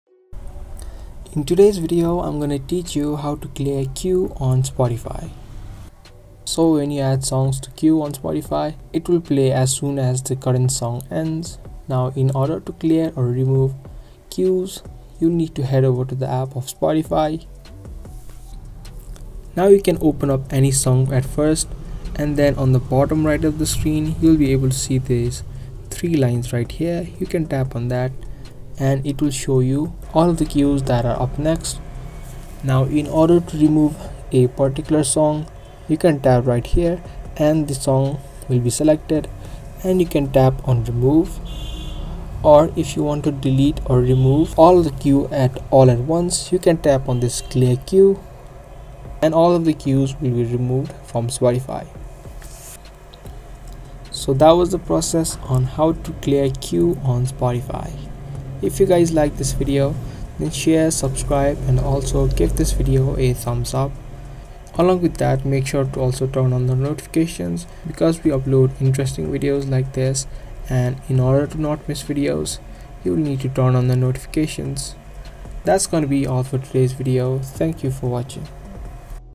How To Clear Your Queue on Spotify App (Quick & Easy!)
Learn how to delete Spotify queue and clear queue on Spotify app.
This video guides you through the exact steps on how to clear Spotify queue on android.